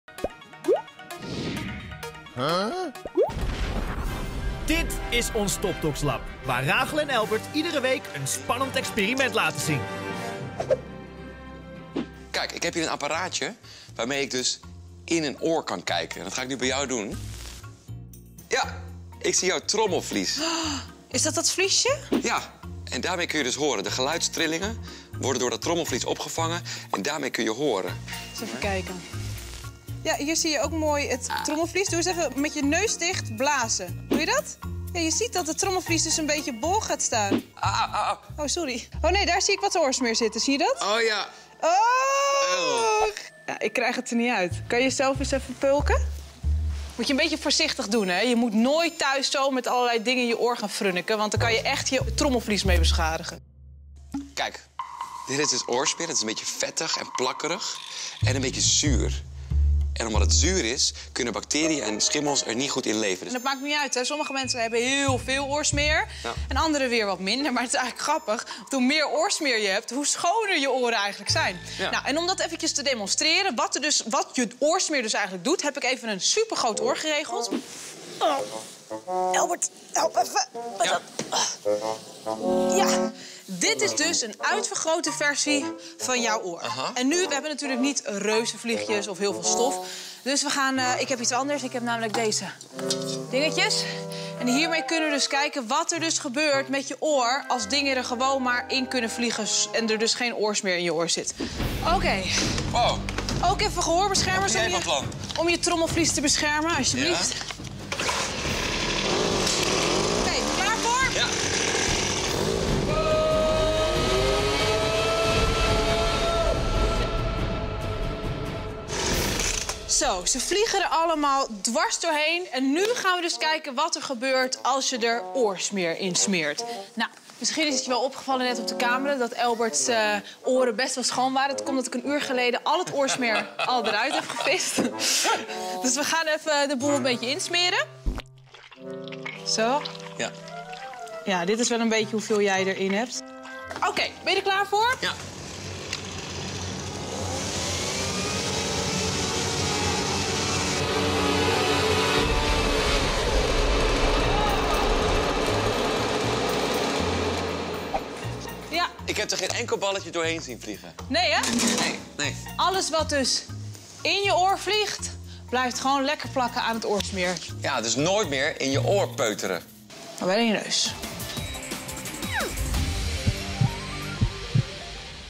WAAROM HEB JE OORSMEER?! - LAB EXPERIMENT
Bah oorsmeer, maar waarom heb je het eigenlijk? In dit lab experiment laten we zien waarom je het nodig hebt!

Wil je meer Topdoks?